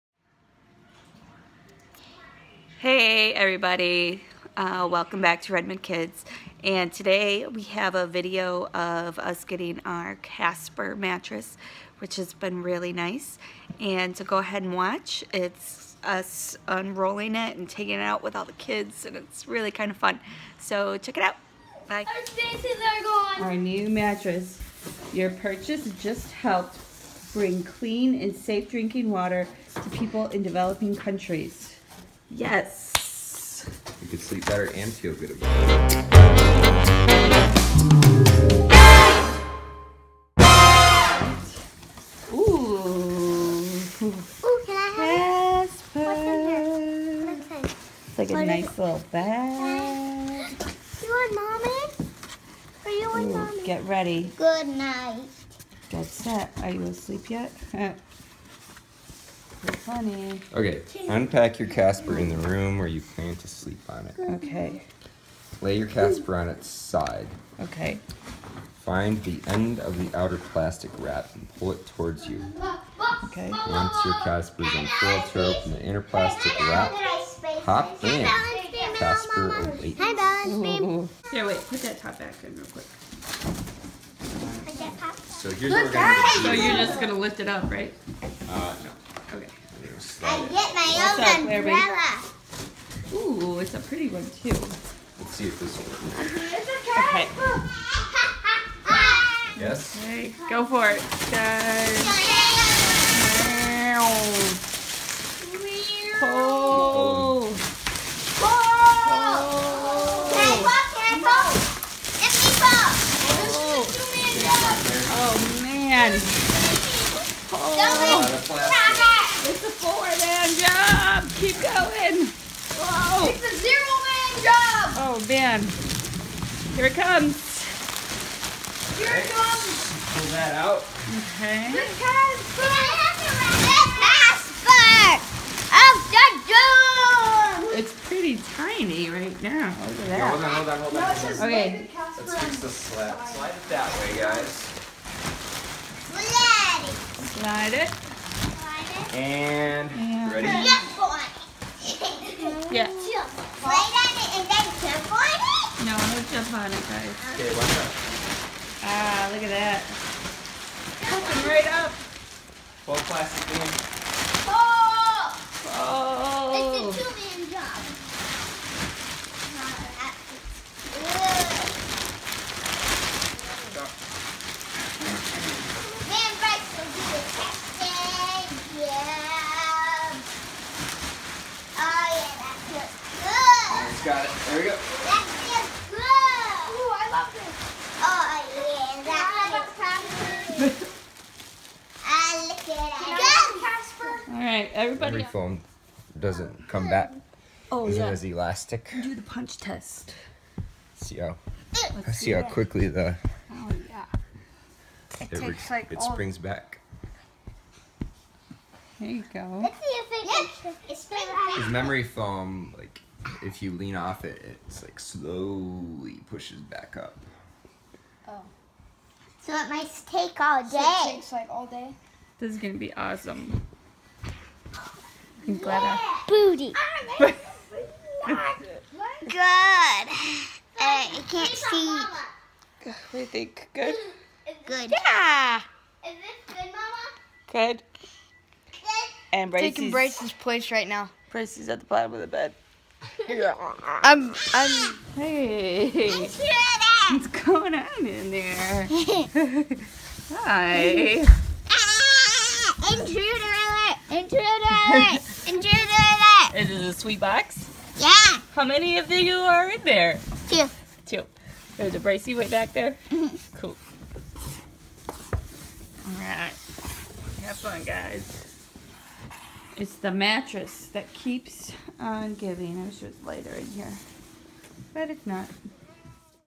Casper Mattress Unboxing and Review Family Style | Vlogmas Day 11
Watch our Casper Mattress Unboxing and review, family style!

We were skeptical about purchasing a mattress online without laying on it first.  We did our research and decided that the Casper Mattress was were we wanted to invest our money.  Their return policy is GREAT.  Casper shipping is SUPER fast.  The customer service I received was fun and easy to communicate with.  The pricing seems fair and the reviews were great.  (Like I said, we did our research...I actually called alot of online mattress companies ON THE PHONE...what?!?!?!?!?!)

Casper Mattress made it easy for us to purchase, unbox and set up our new lovely place to sleep. It's so HUGE. We went from a queen to a king sized bed. I can barely tell when someone (or a pet) is in the bed with me. Ahhhh. the room is nice. I'm so happy it's here, I'm so happy to sleep. Because sleep is good.

Here is a list of the products we purchased for our new bed situation. Get it yo!

Casper King Size Bed:

Bed Frame - King Size

Mattress Protector - King Size

Bed Sheets - King Size

Bed Blanket- King Size